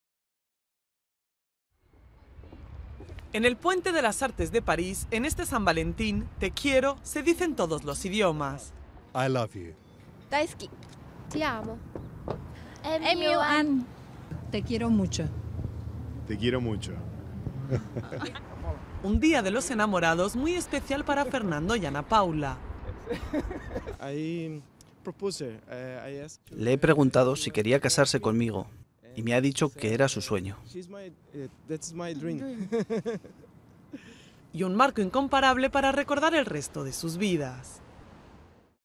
El día de los "Te Quiero"
En el Puente de las Artes de París en este San Valentín "Te Quiero" se dice en todos los idiomas.